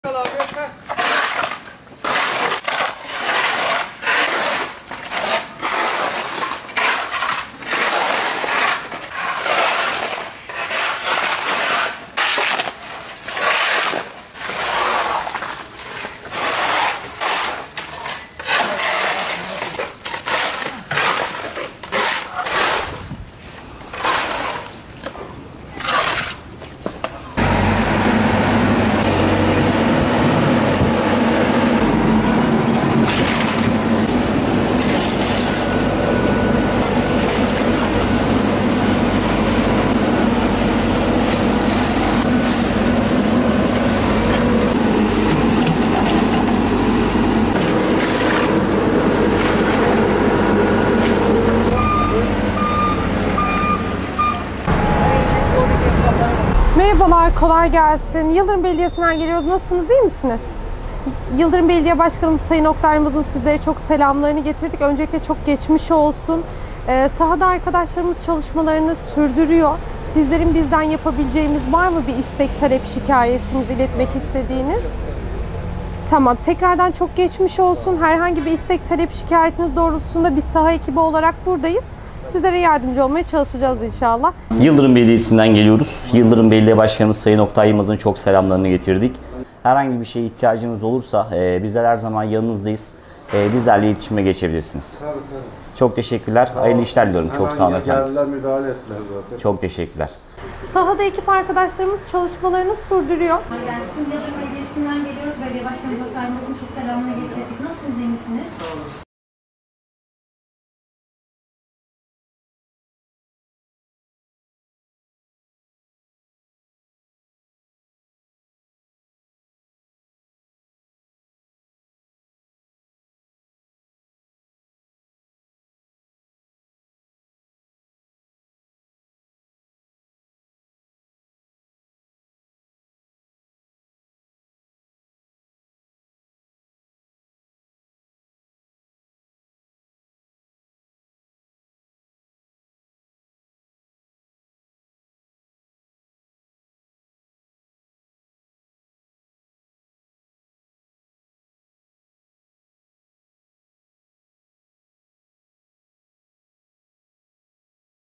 Yıldırım'da yağmur mesaisi
Yıldırım Belediyesi Bursa’da Etkili olan yağışla birlikte ilçe sakinlerinden gelen taleplere anında müdahale etti.
BURSA (İGFA)